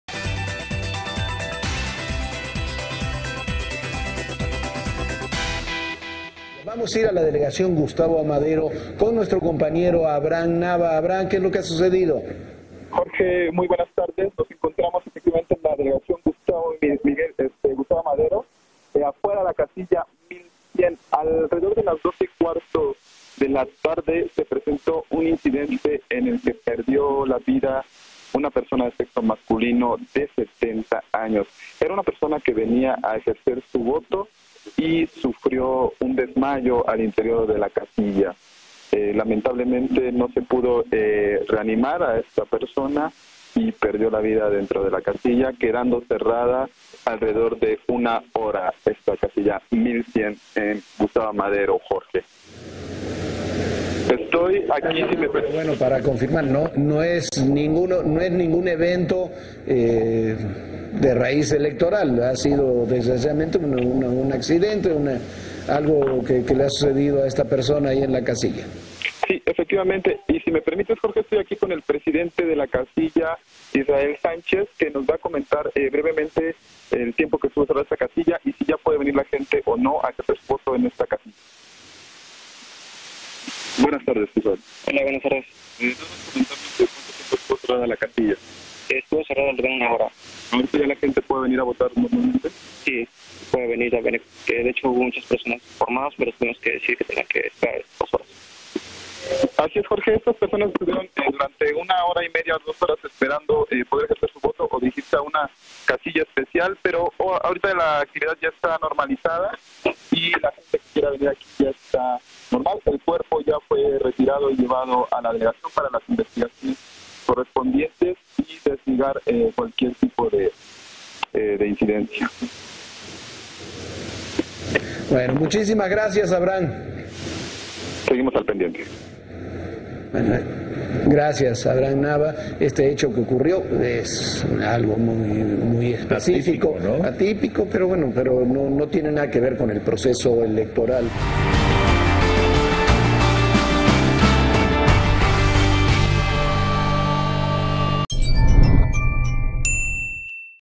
Muere una persona en casilla de la delegación GAM / Elecciones 2015
En un incidente aislado en una casilla ubicada en la delegación GAM una persona perdió la vida.

07 de junio 2015